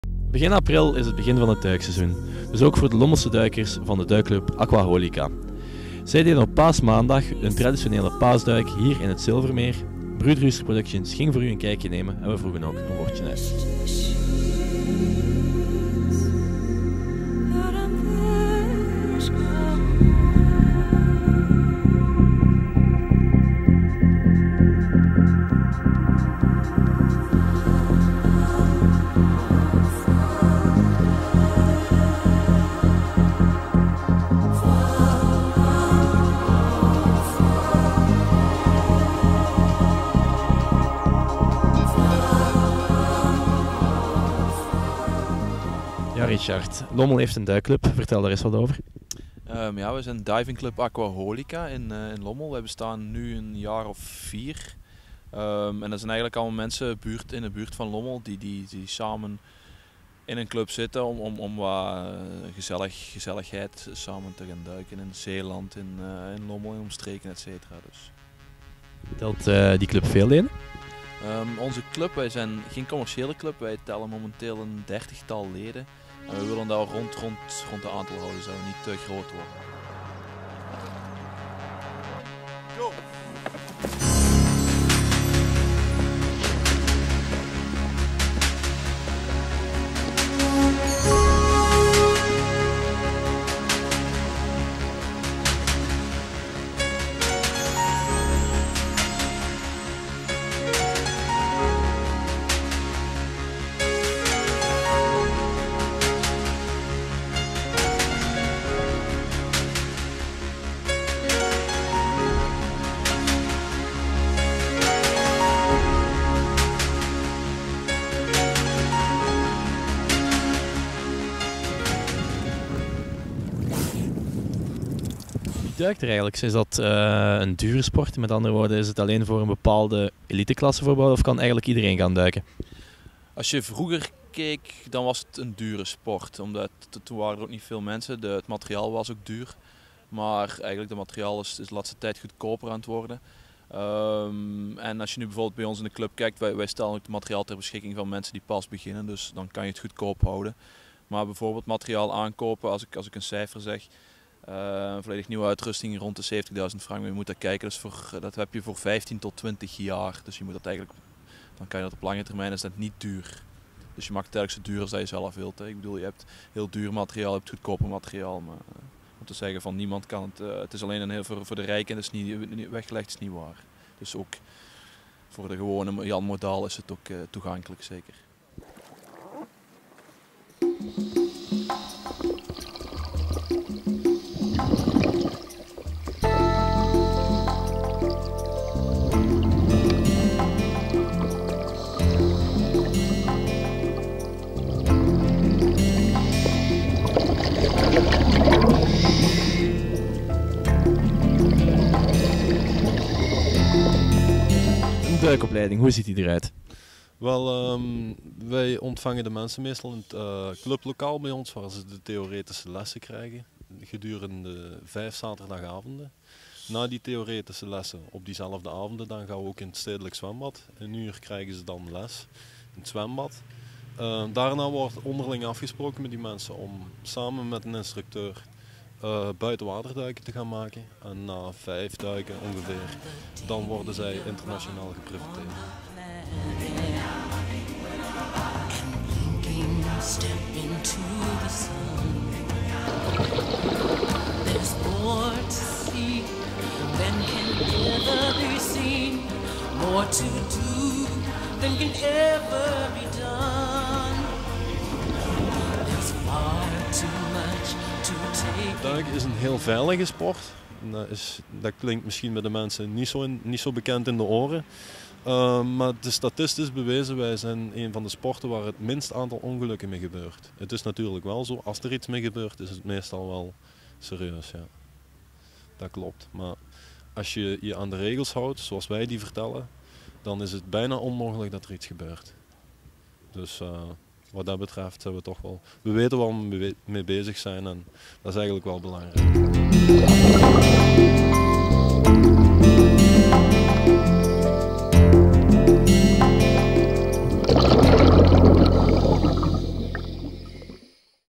Duikclub Aquaholica - Paasduik
Duikclub Aquaholica organiseert naar goede traditie op paasmaandag haar paasduik in het Zilvermeer. Bruudruuster ging voor TVlokaal een kijkje nemen.